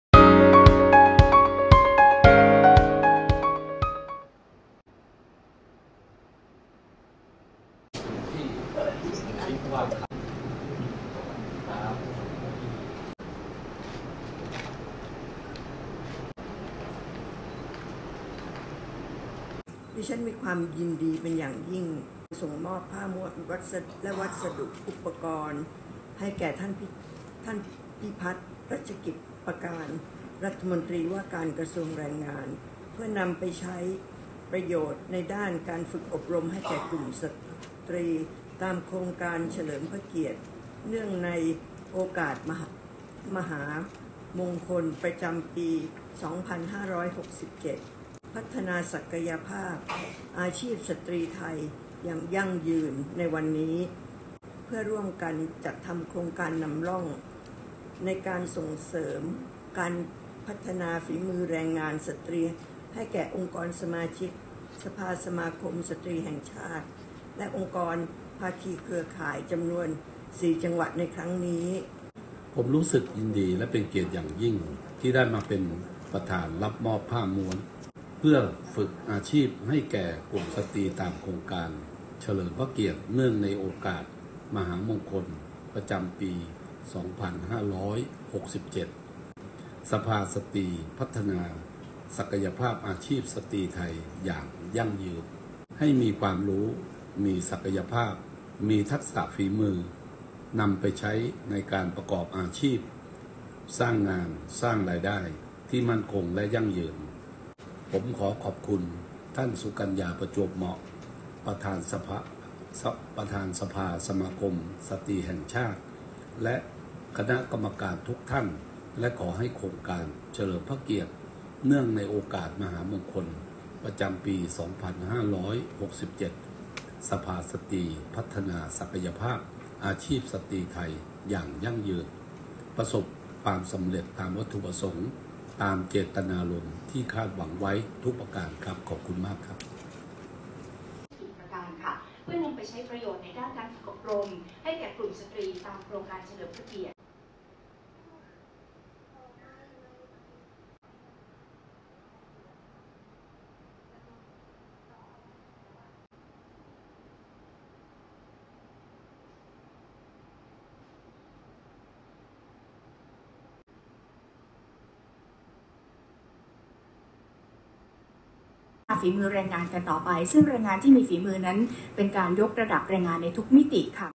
รมว.พิพัฒน์ รับมอบผ้าม้วนและอุปกรณ์จากสภาสมาคมสตรีฯ ส่งเสริมให้สตรีไทยมีอาชีพมั่นคง รายได้ที่ยั่งยืน
วันที่ 23 กุมภาพันธ์ 2567 นายพิพัฒน์ รัชกิจประการ รัฐมนตรีว่าการกระทรวงแรงงาน รับมอบผ้าจากนางสาวสุกัญญา ประจวบเหมาะ ประธานสภาสมาคมสตรีแห่งชาติ ในพระบรมราชินูปถัมภ์ ภายใต้โครงการ “เฉลิมพระเกียรติเนื่องในโอกาสมหามงคล ประจำปี 2567” เพื่อนำไปใช้ประโยชน์ด้านการฝึกอบรมให้แก่กลุ่มสตรี ตามโครงการพัฒนาฝีมือแรงงานสตรี เพื่อยกระดับศักยภาพวัยแรงงานของกรมพัฒนาฝีมือแรงงาน ณ ห้องจัตุมงคล ชั้น 6 กระทรวงแรงงาน

นายพิพัฒน์ รัชกิจประการ
รัฐมนตรีว่าการกระทรวงแรงงาน